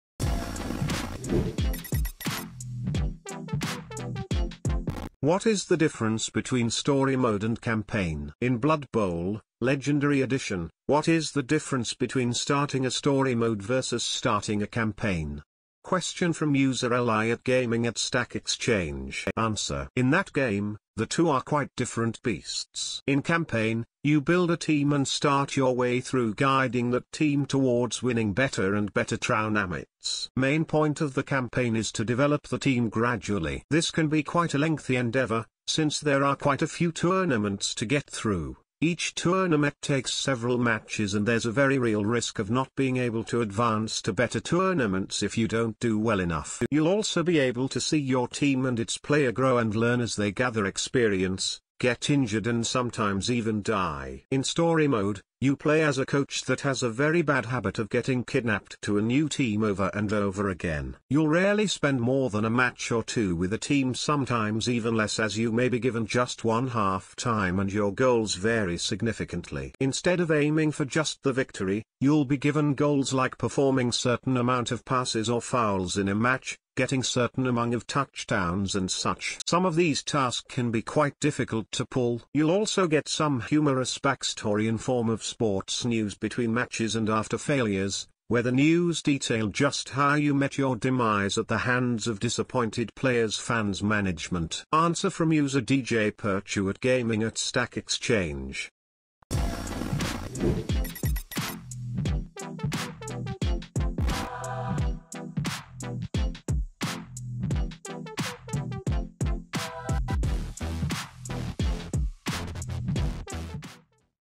What is the difference between story mode and campaign?
In Blood Bowl, legendary edition, what is the difference between starting a story mode vs starting a campaign?

Question from user l I at gaming.stackexchange.com.